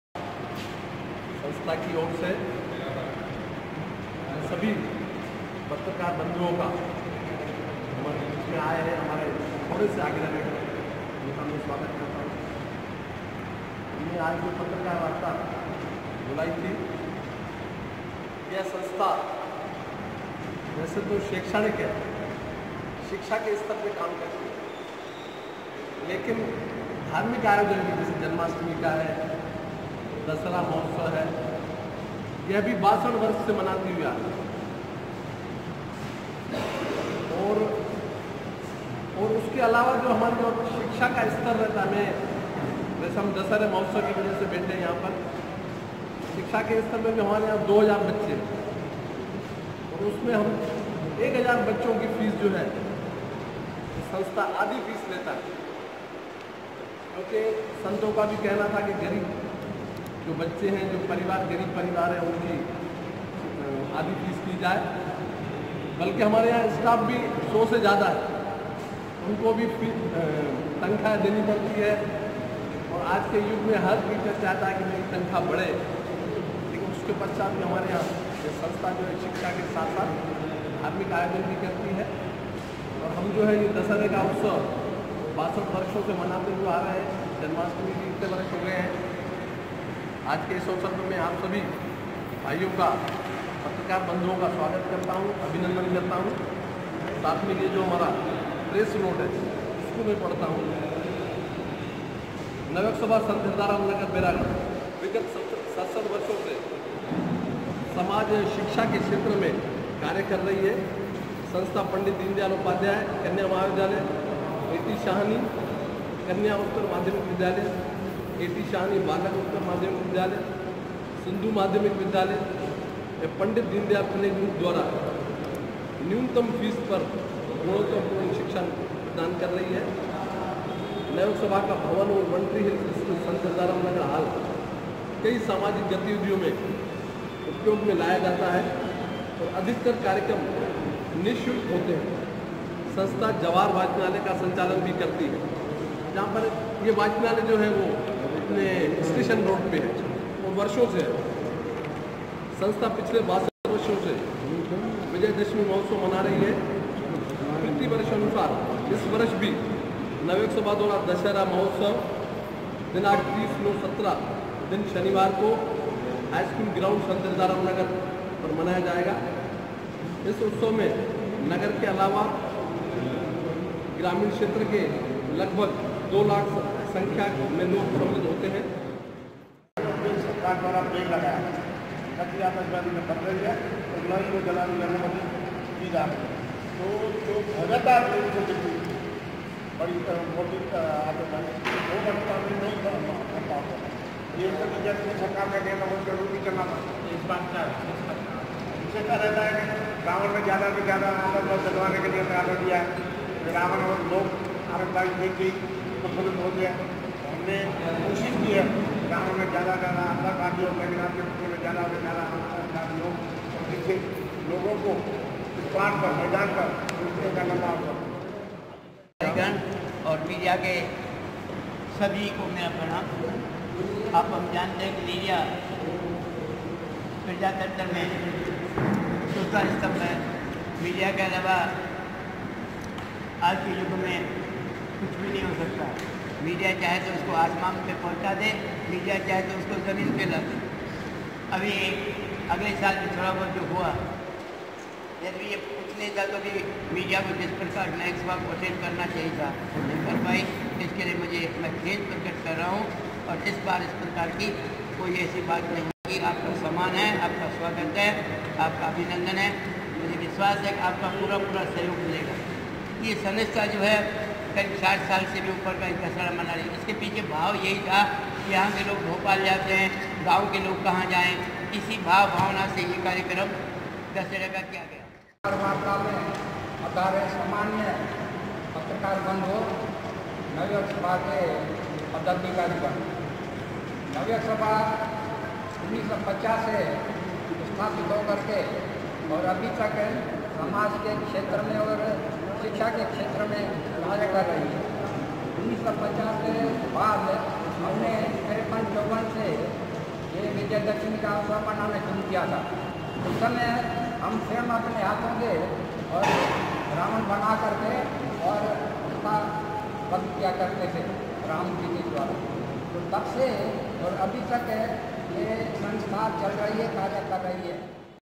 नव युवक सभा दशहरा उत्सव 2017
हर वर्ष की भांति इस वर्ष भी नवयुवक सभा द्वारा जन सहयोग से दशहरा उत्सव मनाने के लिए व उसके पूर्व की तैयारीयो के मदद् नजर पत्रकार वारता व विचार-सलाह के लिए बैठक का आयोजन किया गया ।